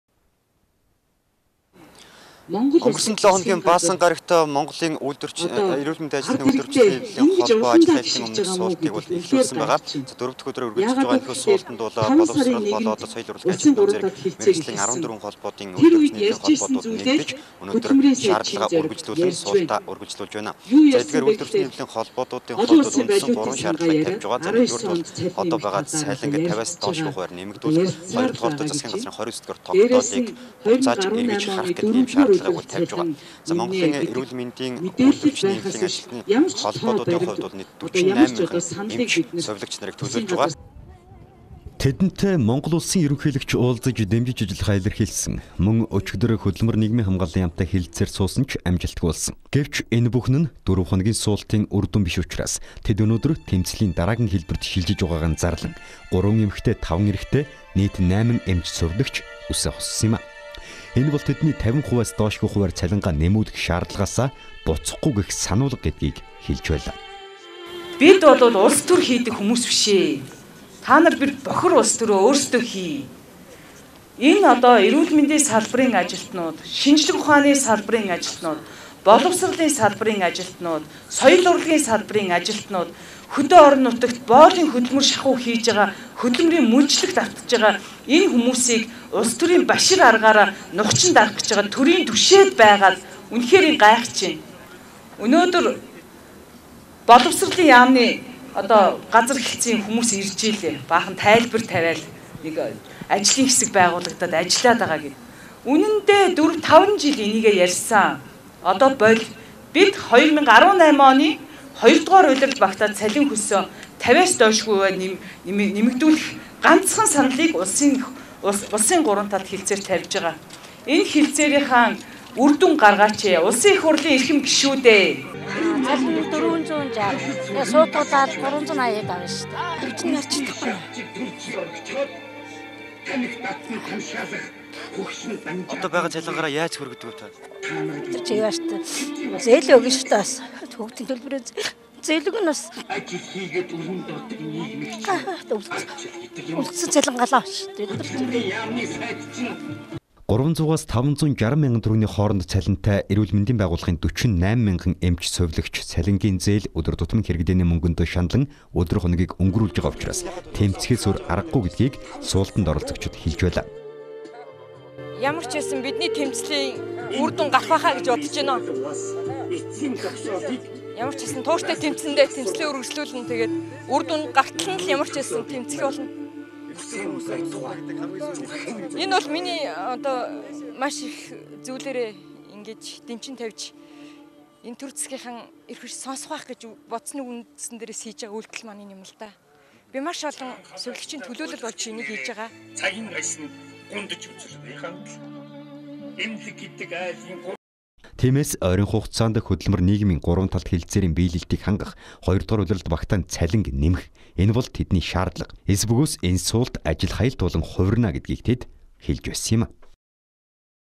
ЭМЧ,ЭМНЭЛГИЙН АЖИЛТНУУД ТЭМЦЛИЙН ДАРААГИЙН ХЭЛБЭРТ ШИЛЖИЖ “ҮСЭЭ ХУСЛАА”
300-560 МЯНГАН ТӨГРӨГНИЙ ХООРОНД БАЙГАА ЦАЛИНГ 2-Р УЛИРАЛД БАГТААН 50-ИАС ДООШГҮЙ ХУВИАР НЭМЭХ ХҮРТЭЛ ТЭМЦЛЭЭ ҮРГЭЛЖҮҮЛНЭ ГЭЛЭЭ